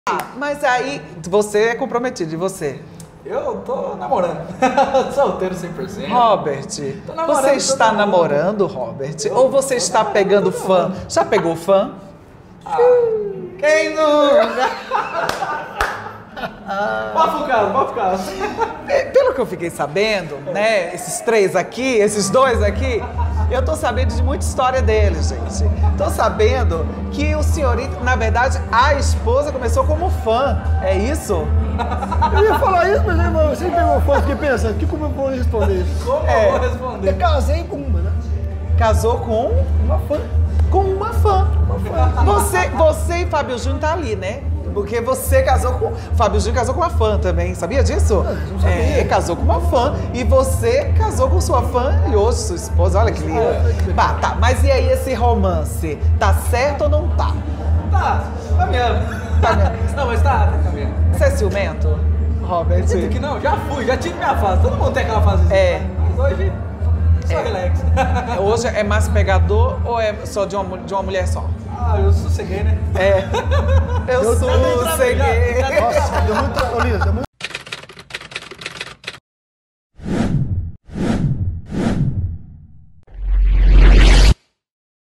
"Casei com uma fã", revela sertanejo Ruan
Seja bem-vindo!

Neste corte o sertanejo Ruan revela que casou com uma fã.